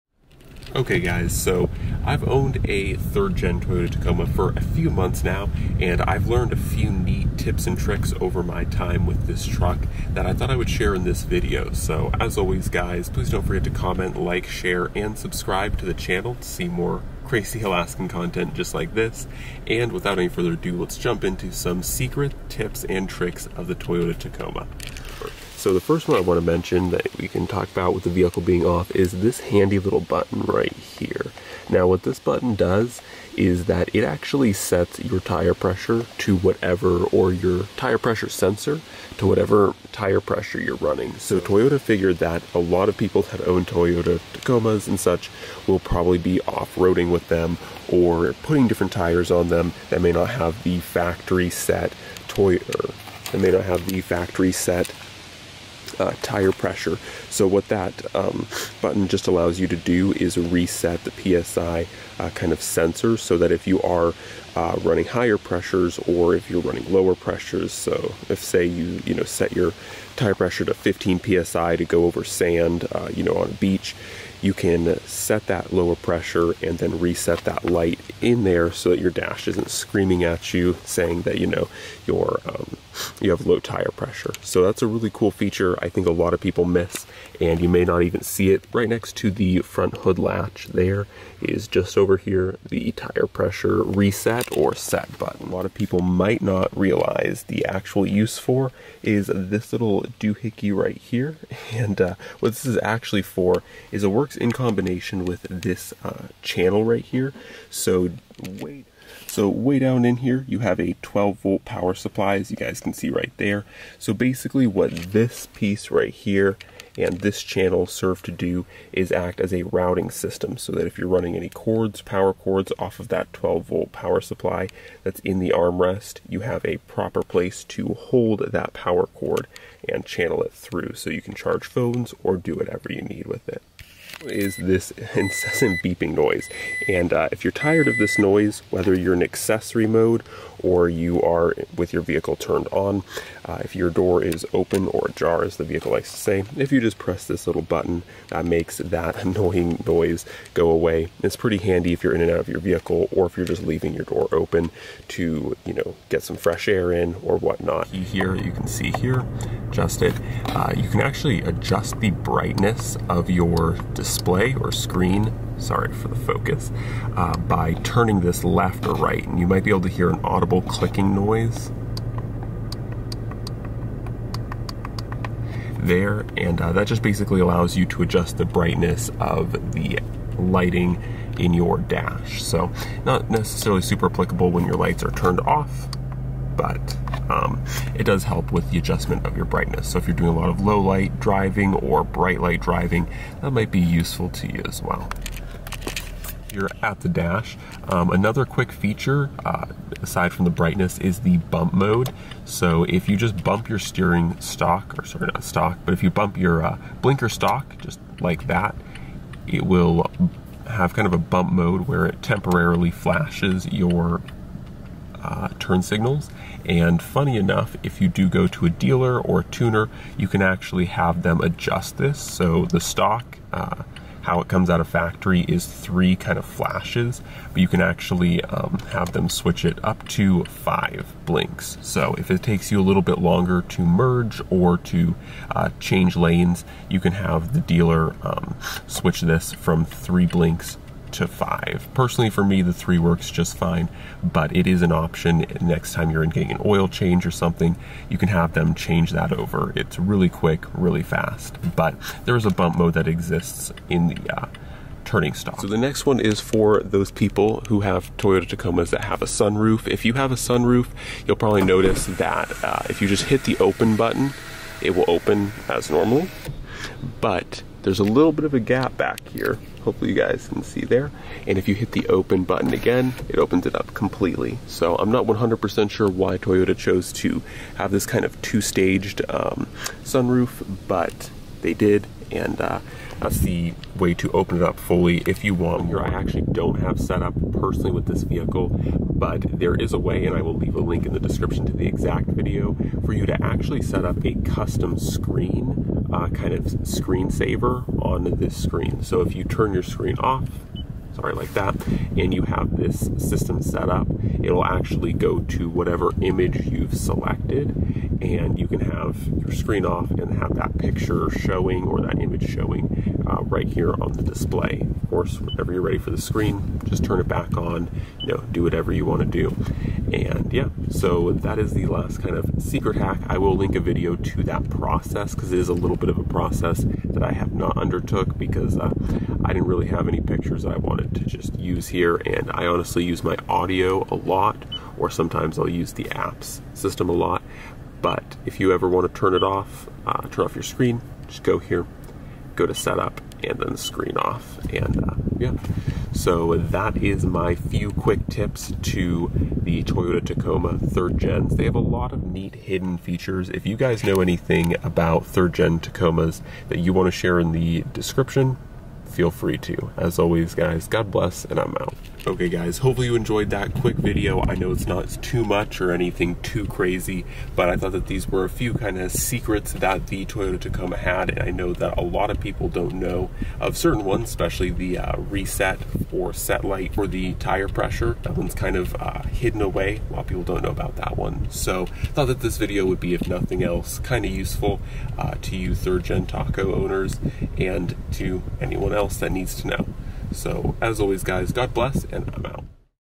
Top 5 Hidden Features of ANY 3rd Generation Toyota Tacoma
For those owners of the legendary 3rd Generation Toyota Tacoma's I thought I would share with you some of the hidden features you might not know of.